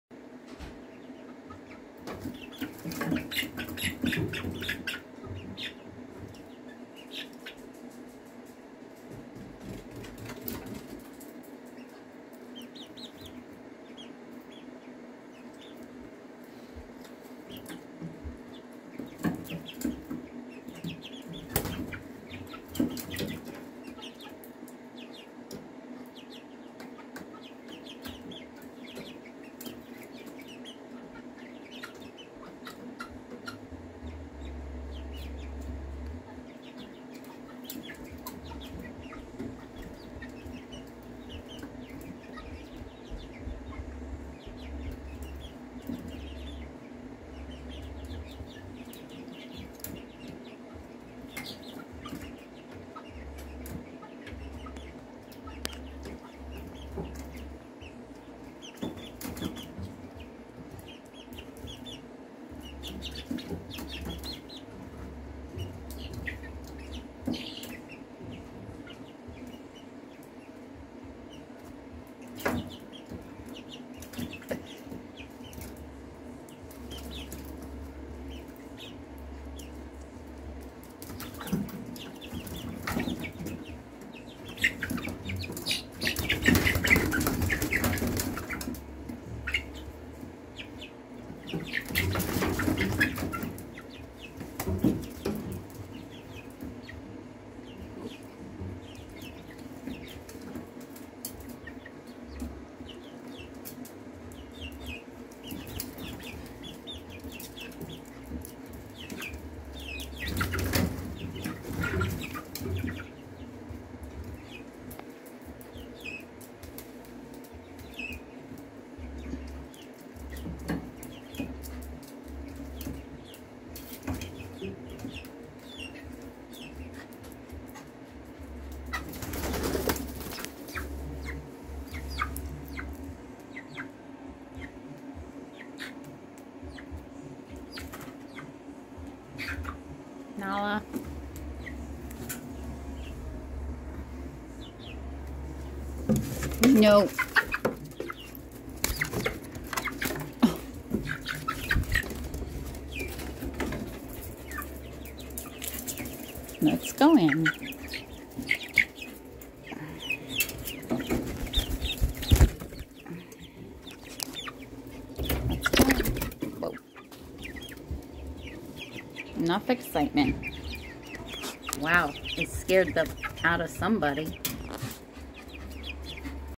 Chicks- almost 8 wks old- want to explore!
They like to fly out, as you see, they don't grip well on the steel bar. Nala has to come and inspect things, and make the chickens a nervous wreck. She must've scared the 💩 out of a few🤣! (wait for it!)